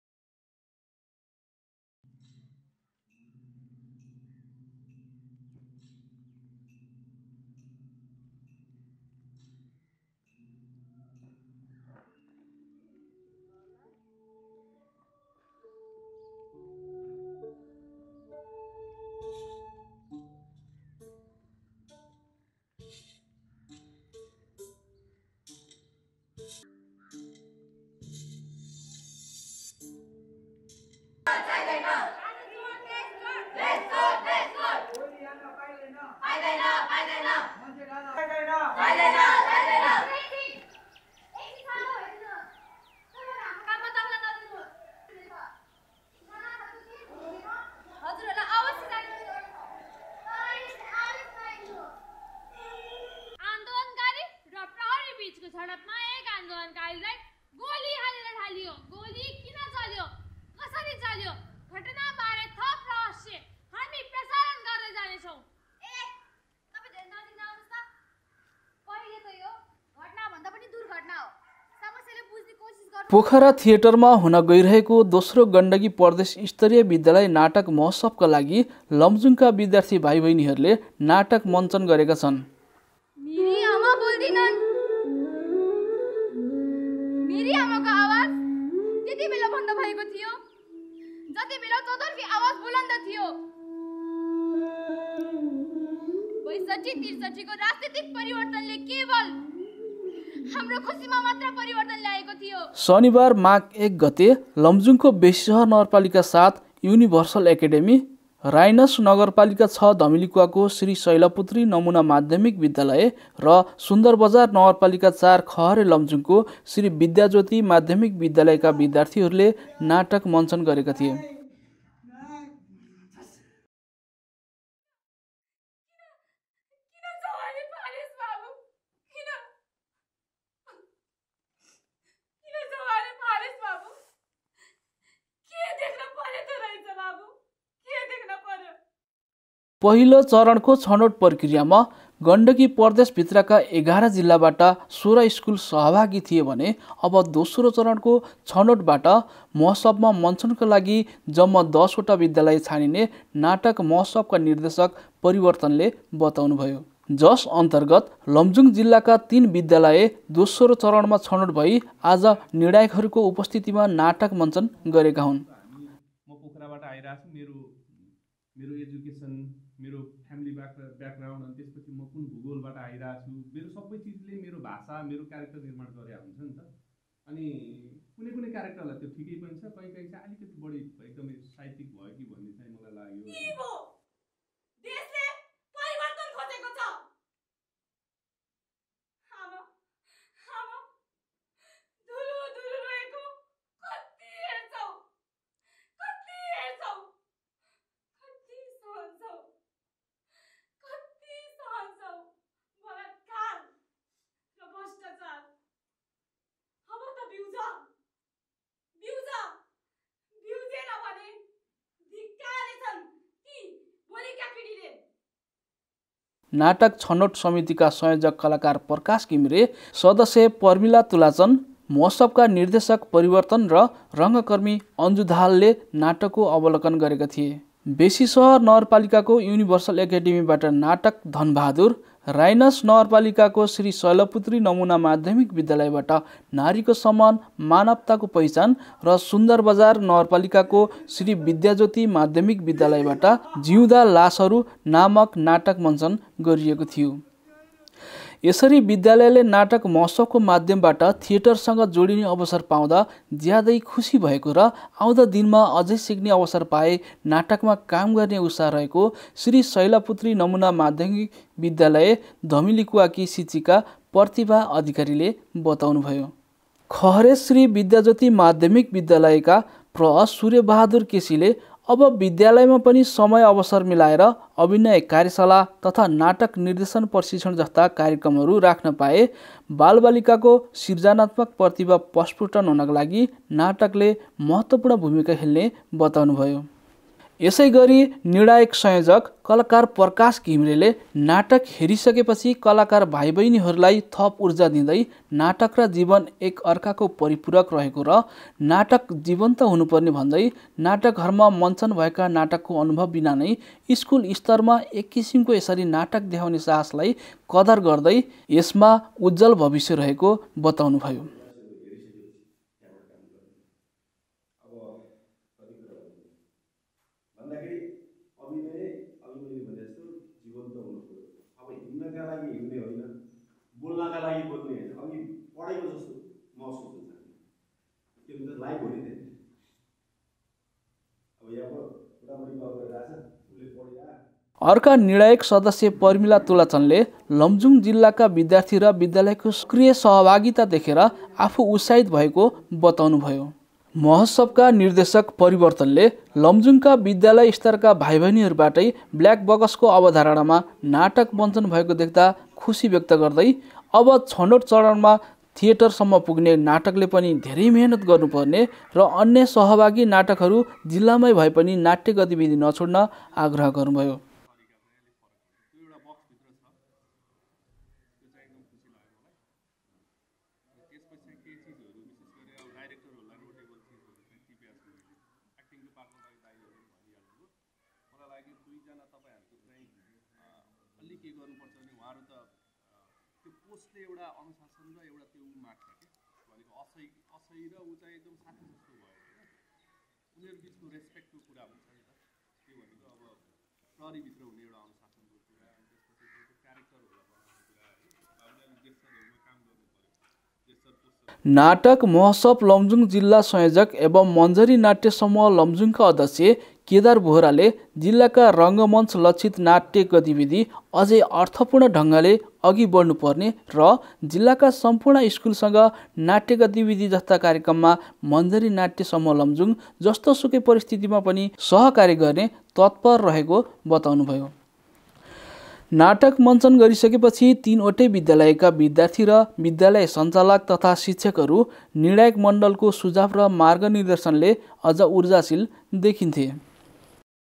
नाटक महोत्सवको तयारीमा जुट्दै लमजुङको तीन विद्यालय Manjari lamjung Dramanews
नाटक महोत्सवको तयारीमा जुट्दै लमजुङको तीन विद्यालय
पोखरा थिएटरमा हुन गइरहेको दोस्रो गण्डकी प्रदेश स्तरीय विद्यालय नाटक महोत्सवका लागि लमजुङका विद्यार्थीहरुले नाटक मञ्चन गरेका छन् ।